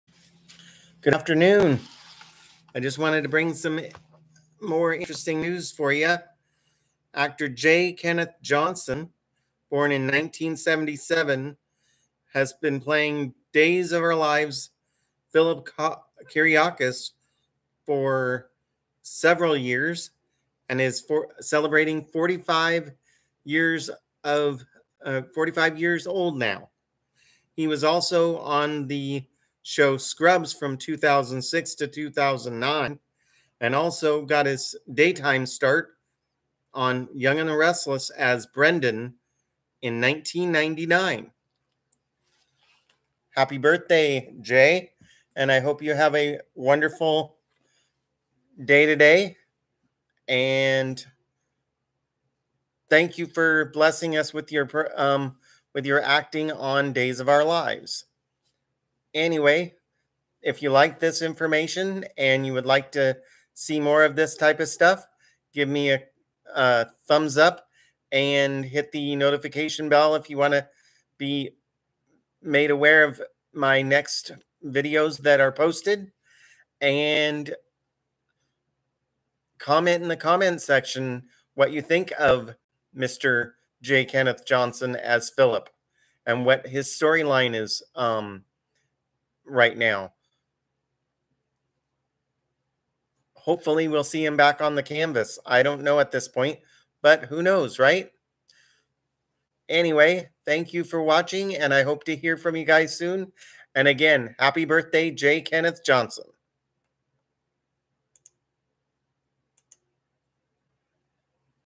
Actor J. Kenneth Johnson is celebrating his 45th birthday today, February 24th.
J. Kenneth Johnson has been playing Philip Kiriakis off and on since 1999. He was seen on the medical comedy Scrubs from 2006-09. His first role on daytime soaps was on Young and The Restless as Brendon in 1999.